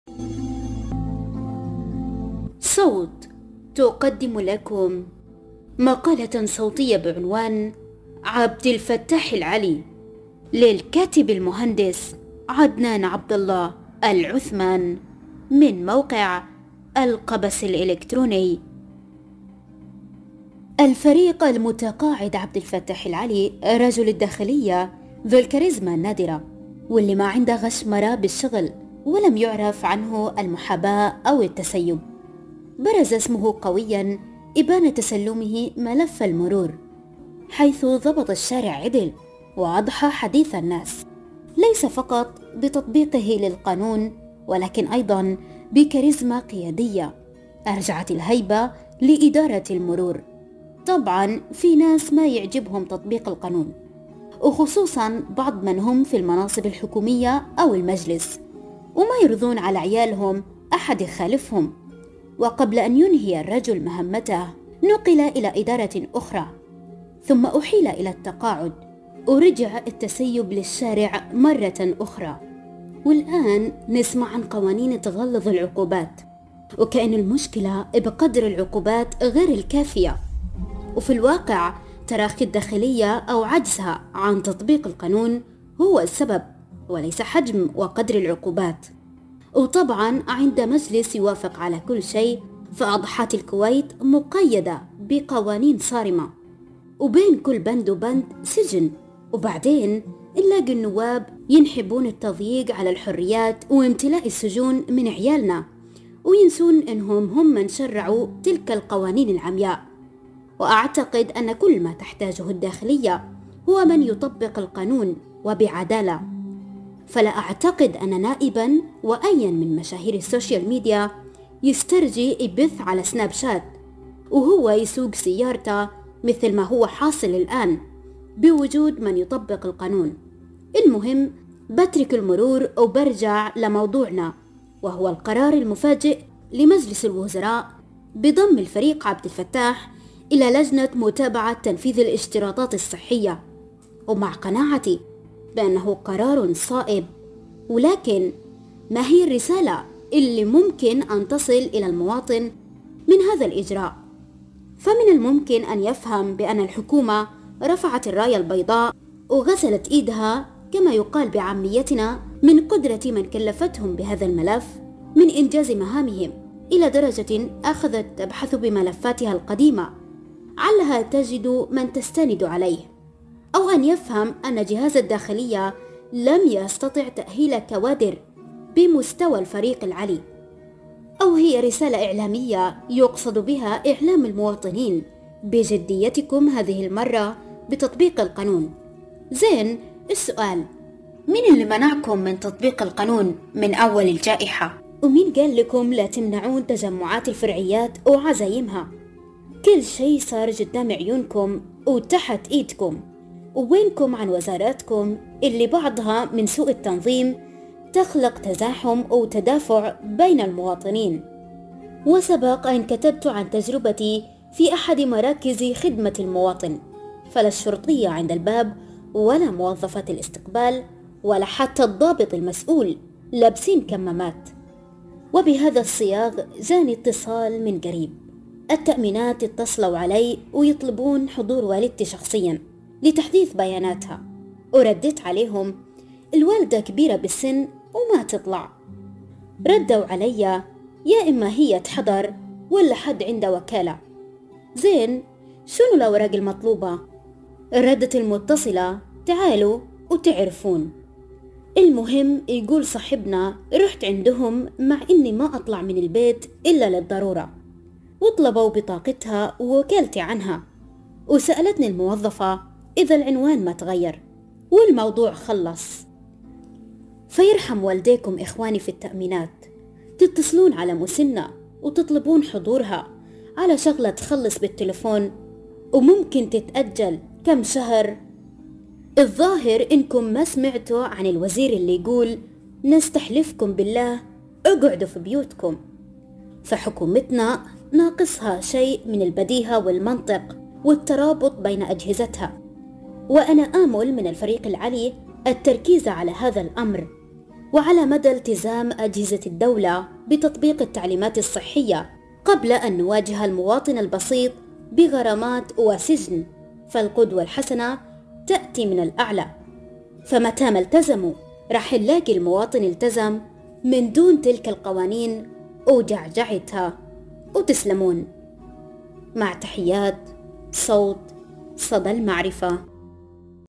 مقال "عبدالفتاح العلي"
مقال_صوتي بعنوان: عبدالفتاح العلي للمهندس عدنان_العثمان في جريدة القبس بتاريخ 2020/10/20